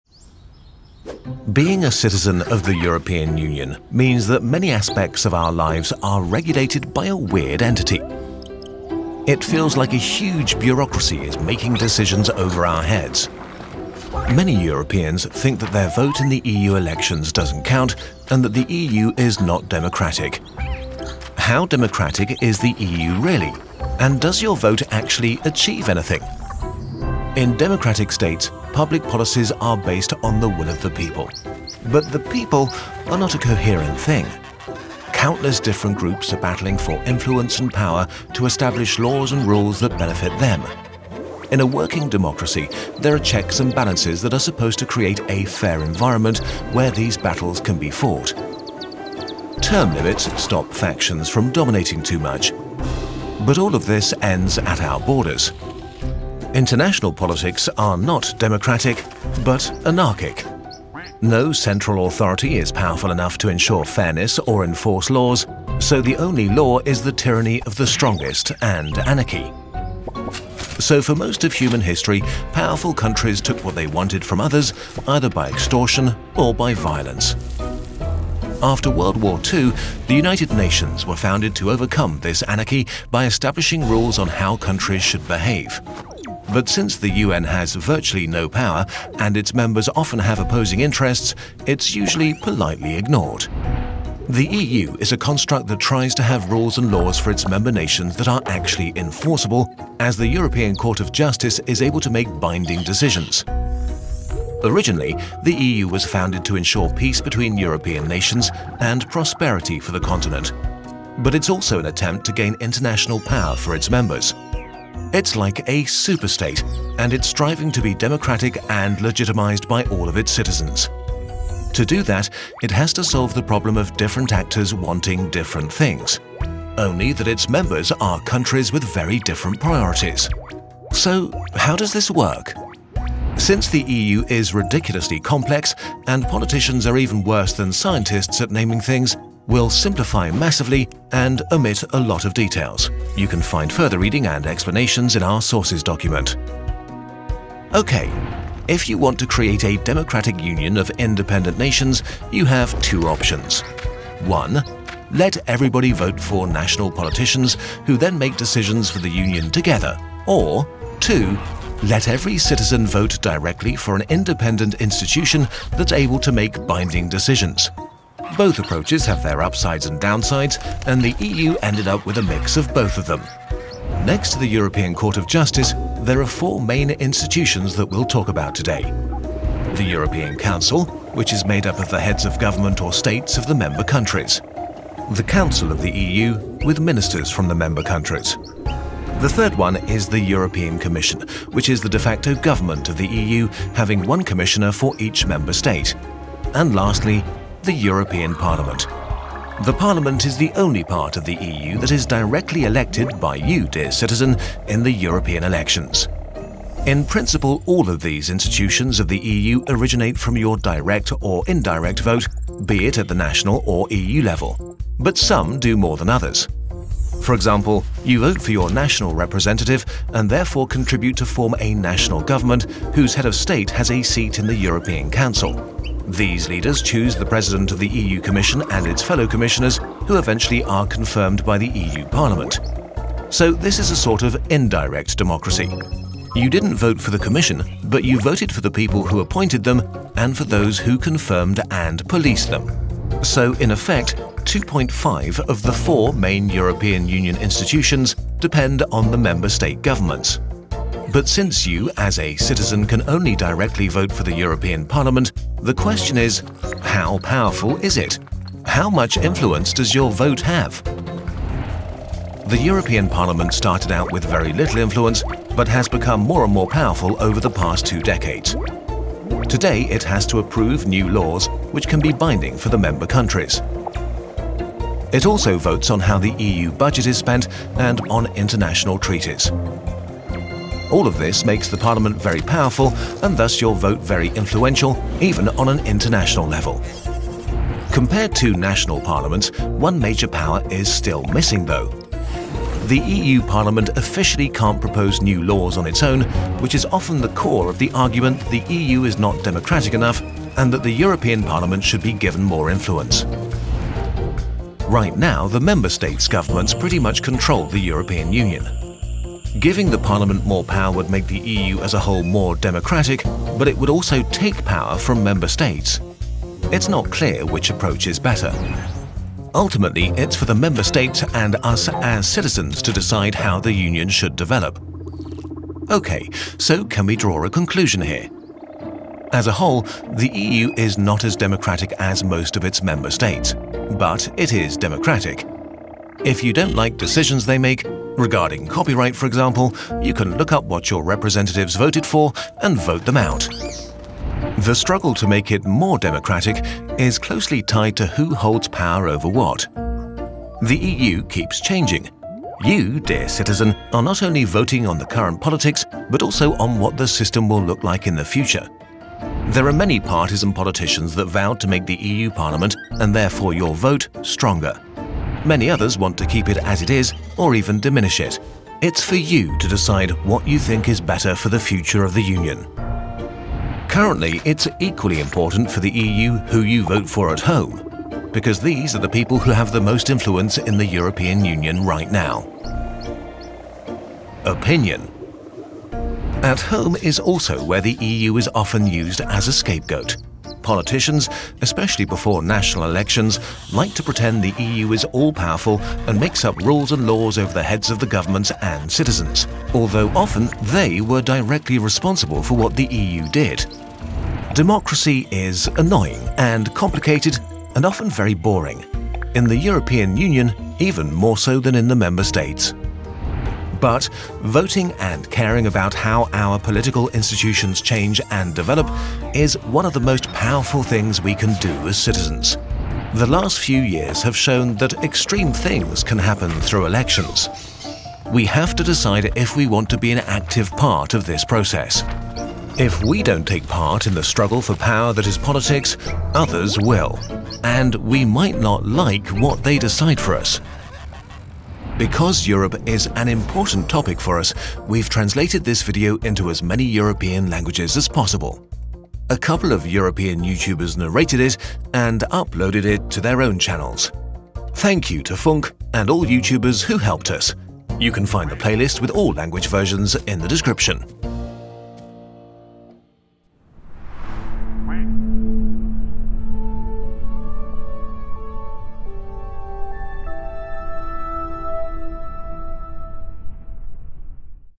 Is the EU Democratic? Does Your Vote Matter?
Thanks to all the YouTubers who supported us!!!

Being a citizen of the European Union means that many aspects of our lives are regulated by a weird entity. It feels like a huge bureaucracy is making decisions over our heads. How democratic is the EU really and does your vote actually affect anything?